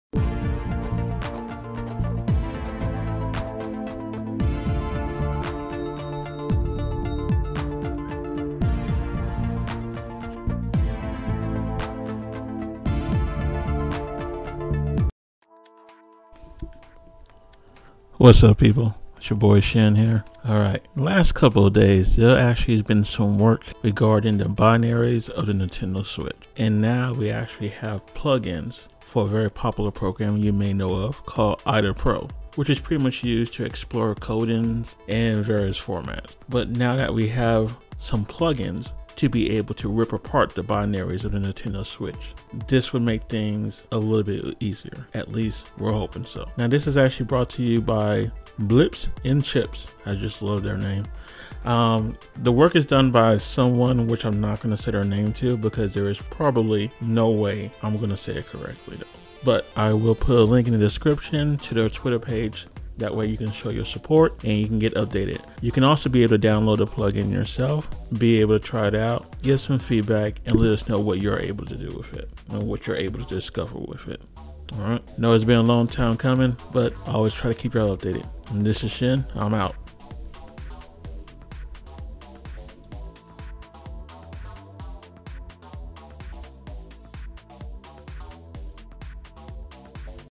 Hacking the Nintendo Switch : Part XVI : Ripping Binaries
Binary means composed of two pieces or two parts and may refer to different things in different worlds of Mathematical, Computing, Science and Others.

But, in Computing, Binary refers to :

Binary file, composed of something other than human-readable text
Executable, a type of binary file that contains machine code for the computer to execute

Binary code, the digital representation of text and data.

♬ Music ♬
Artist / Title